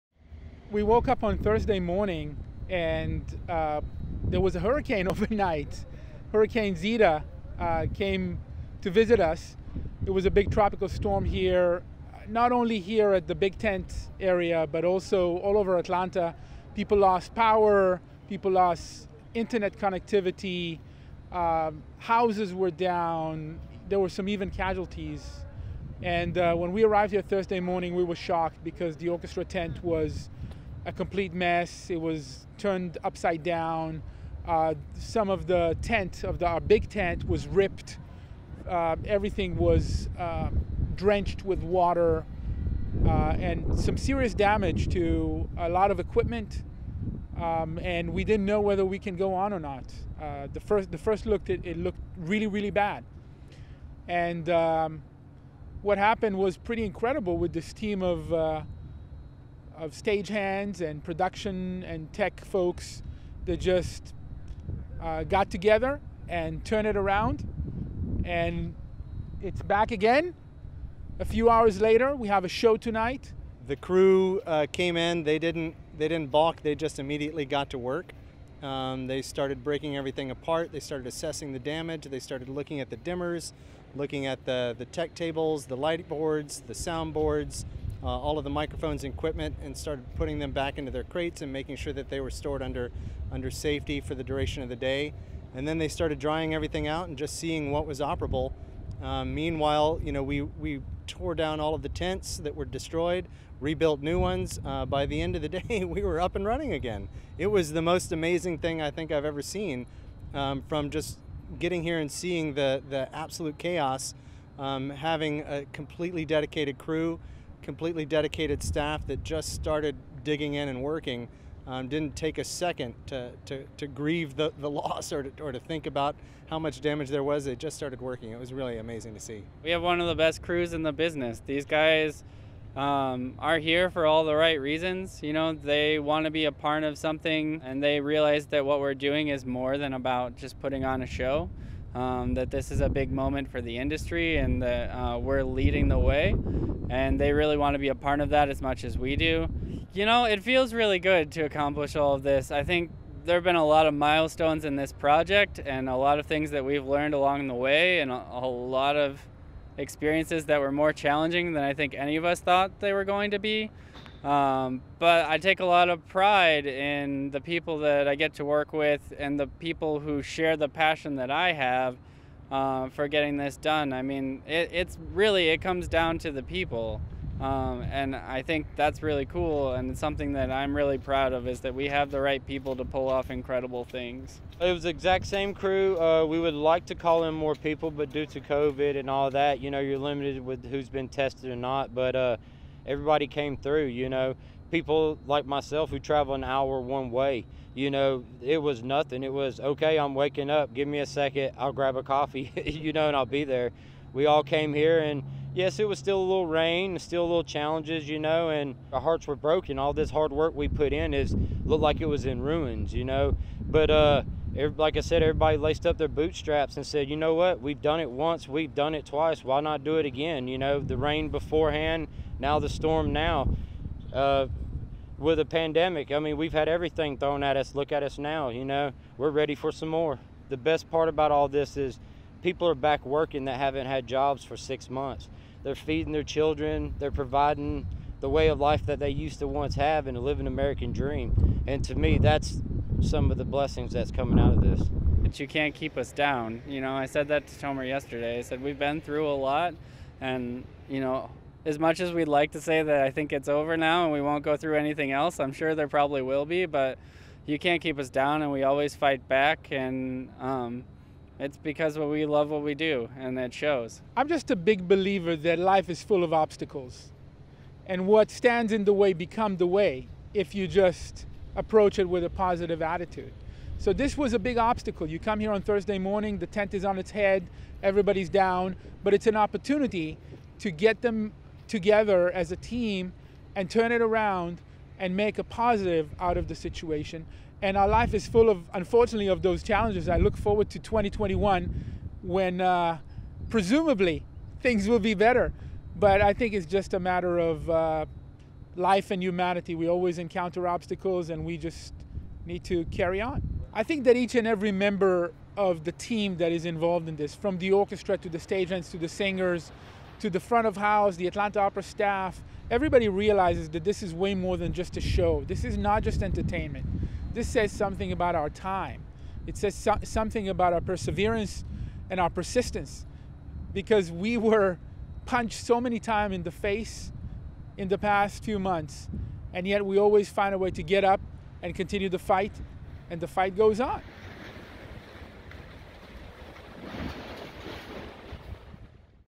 Hurricane Zeta Aftermath at the Big Tent
the Big Tent may have taken a hit from Zeta on Thursday, but the hardworking Atlanta Opera production team and IATSE LOCAL 927 got to work immediately, assessing and repairing the damage brought on by high winds and rain.

The show goes on.